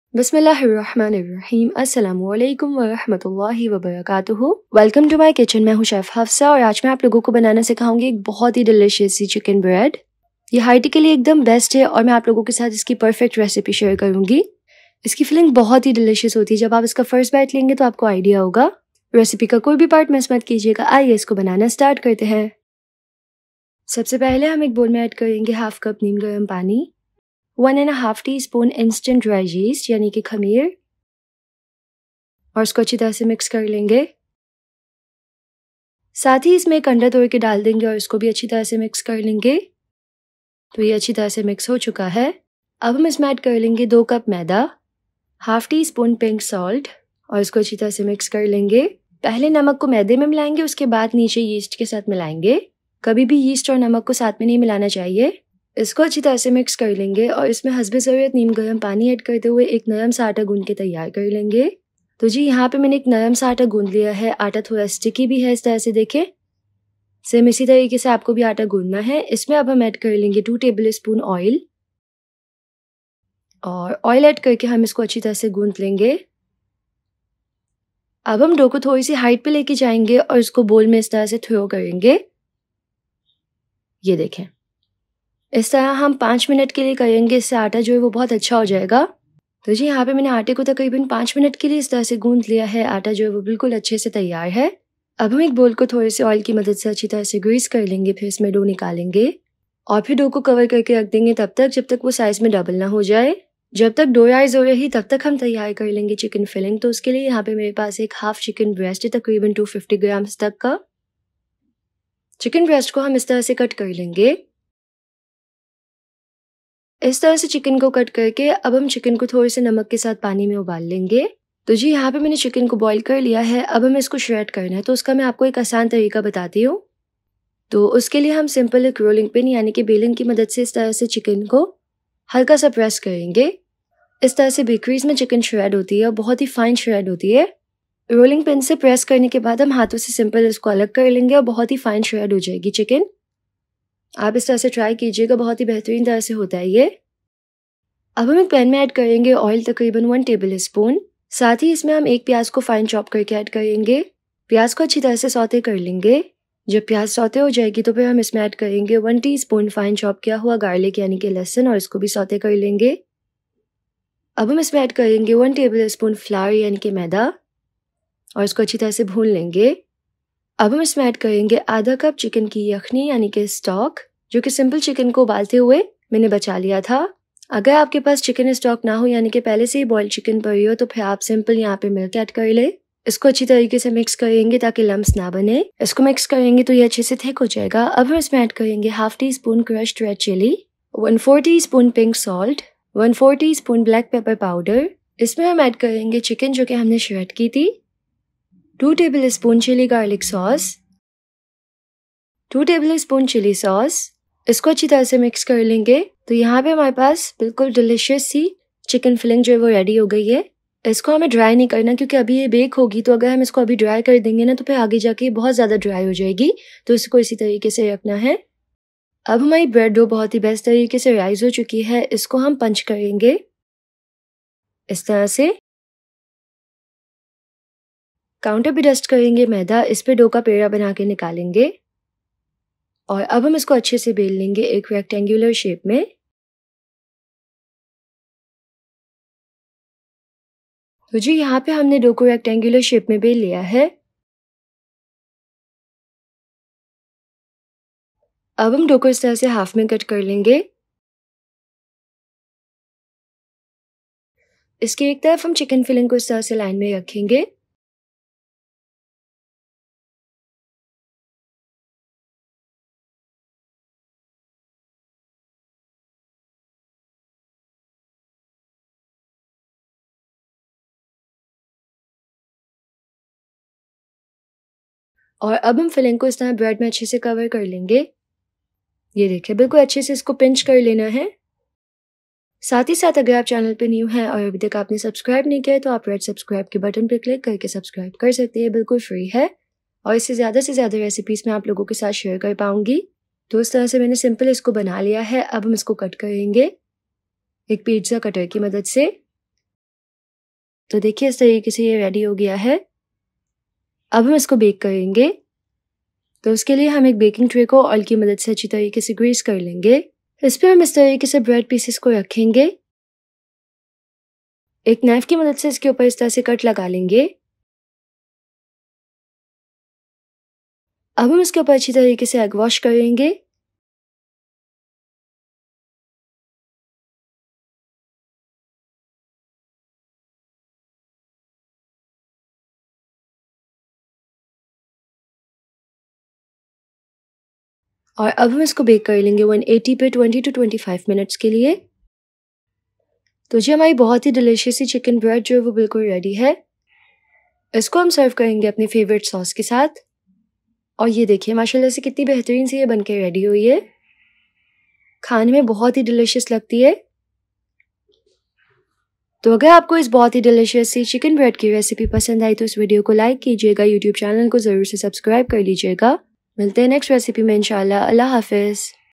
Chicken Bread Recipe 😍 By Chef Hafsa | Perfect for High Tea Snack
Today you'll learn how to make Chicken Bread Recipe 😍 By Chef Hafsa | Perfect for High Tea Snack